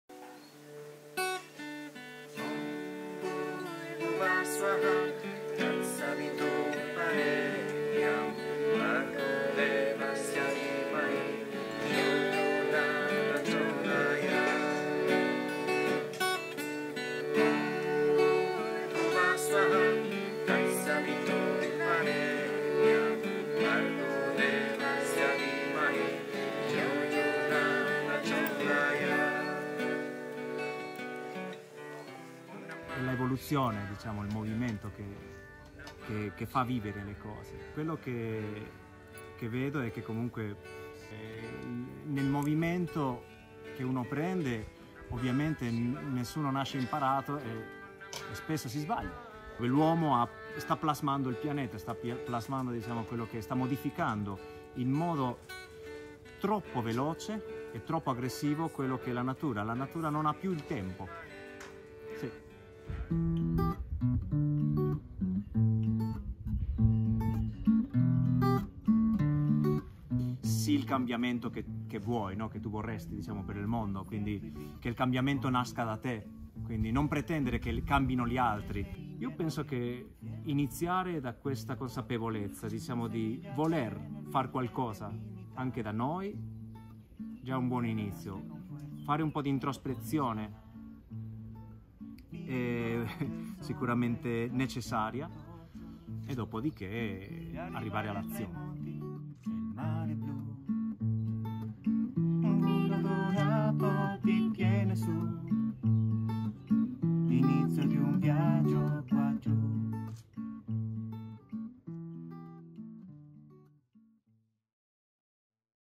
Weekend con Sergio Muniz e Morena Firpo @ Oasi Dyamo
Condividere l'amore per la Natura, interrogarsi sui suoi misteri e sulla sua bellezza, sentirci parte di un tutto, capire insieme come conservarla e salvaguardarla: è questo lo spirito dei nostri 'Weekend con...'!
Riviviamo insieme i momenti più belli vissuti con il nostro ultimo ospite Sergio Muniz che ci ha tenuto compagnia insieme a Morena Firpo.
Non perdetevi il prossimo evento Oasi Dynamo, ecco il programma completo
Interviewed by Matteo Pennacchi
video credit: Valentina Bestetti